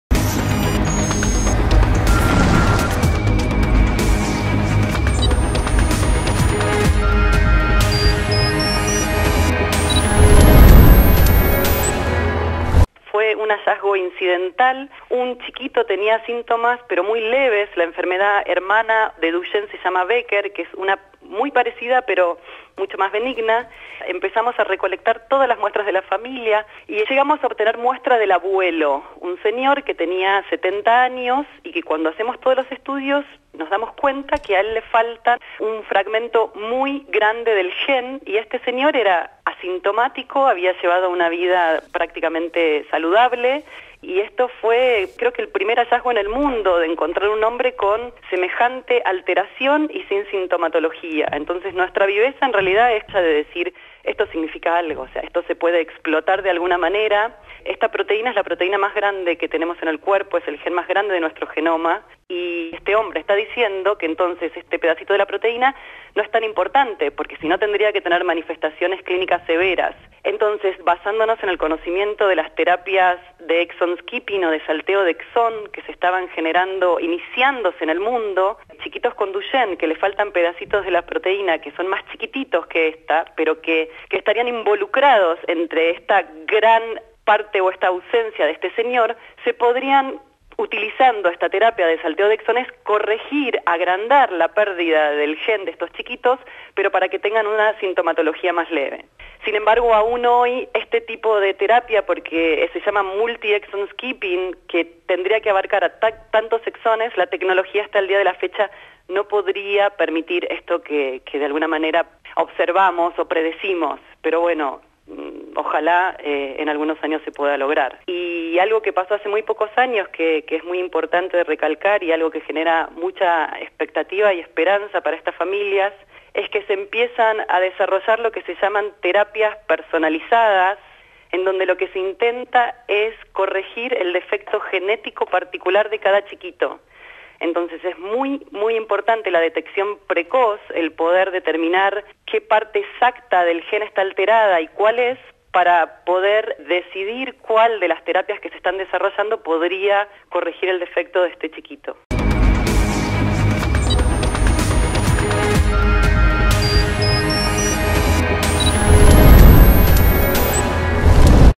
Una esperanza para combatir la distrofia muscular de Duchenne
Es una de las enfermedades genéticas más frecuentes. Si bien es una utopía pensar hoy en una cura, varias líneas de investigación a nivel mundial permiten imaginar el surgimiento de tratamientos eficaces para enfrentar esta grave enfermedad.